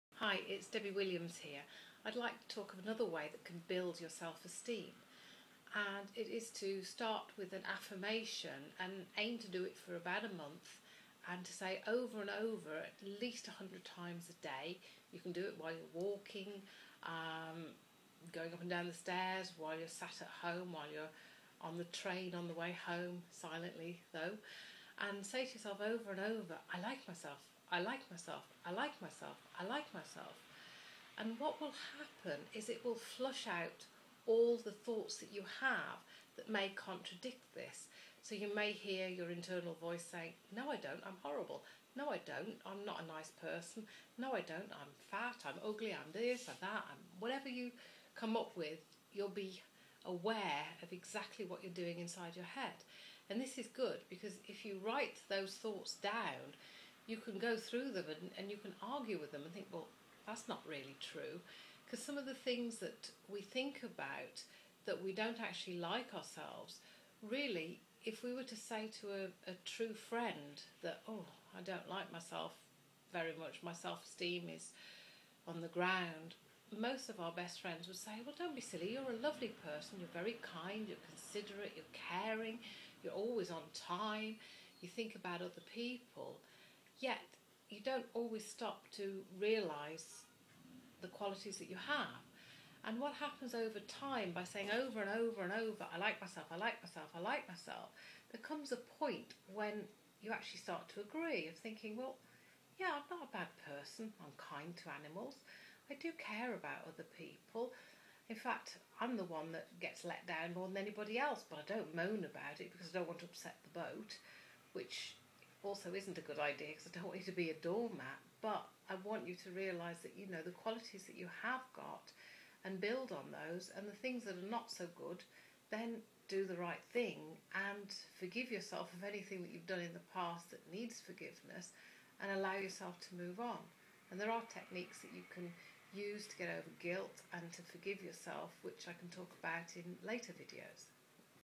Low Self Esteem and liking yourself 2
Low Self esteem is something you can change with hypnosis and NLP to have much higher self esteem and belief in yourself